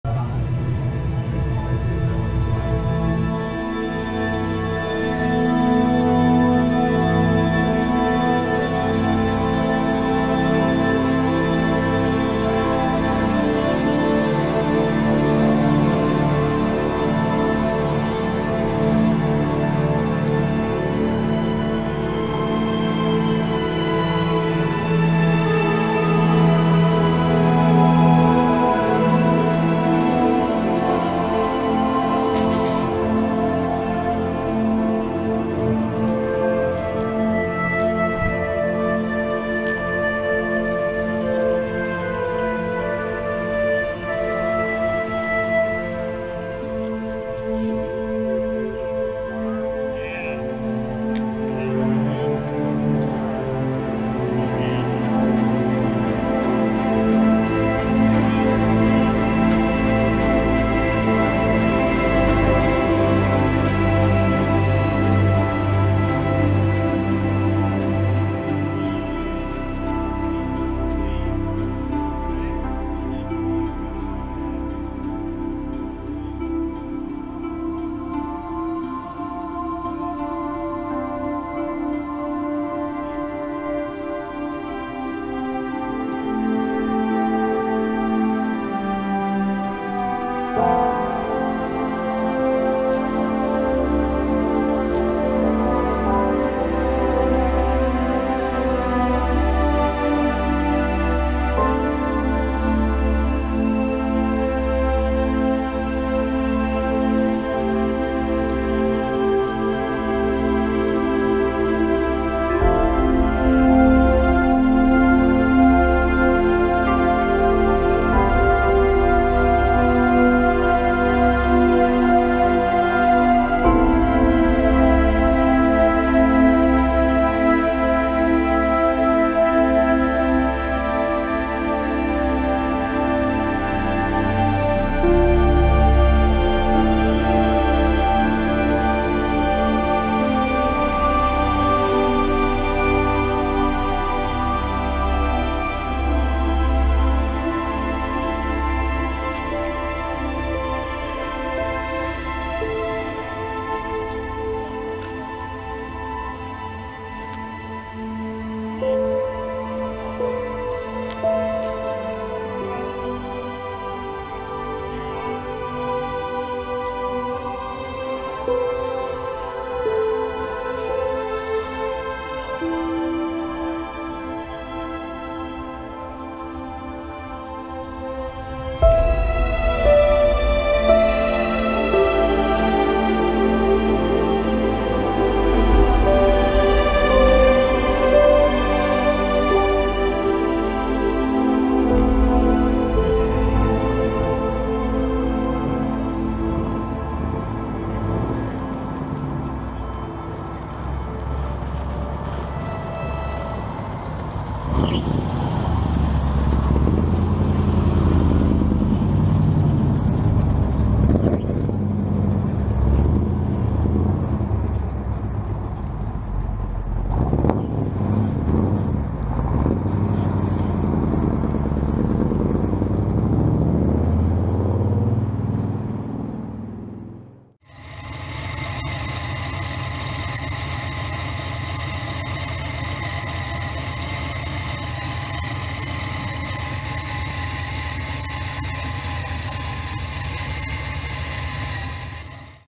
1959 Chevrolet Impala at Vescio's Customs in Rogers Minnesota
1959 Chevrolet Impala George Barris Tribute at Vescio's Custom's open house and car show in September of 2024

2024 Vescio's Car Show and Open House Video (1 Hour)